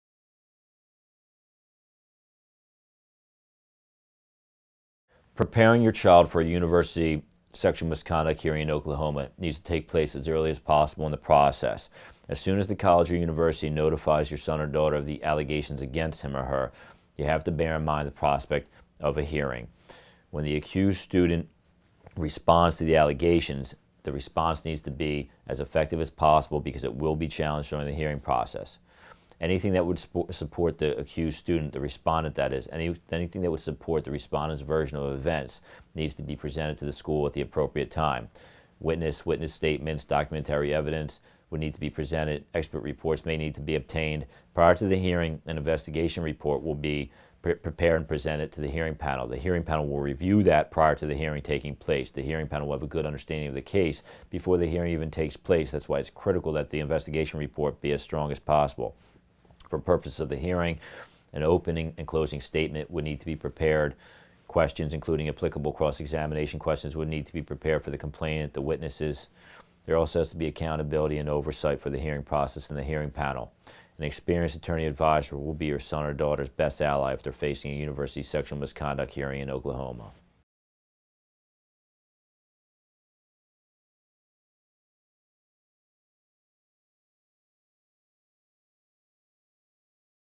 How Can I Help My Child Prepare for a College Sexual Misconduct Hearing in Oklahoma?
Expert attorney answers questions about academic misconduct, Title IX, and other student disciplinary issues. In this video we asked, 'How Can I Help My Child Prepare for a College Sexual Misconduct Hearing in Oklahoma?'.
Ask The Attorney (0:01)
Expert Answer (0:05)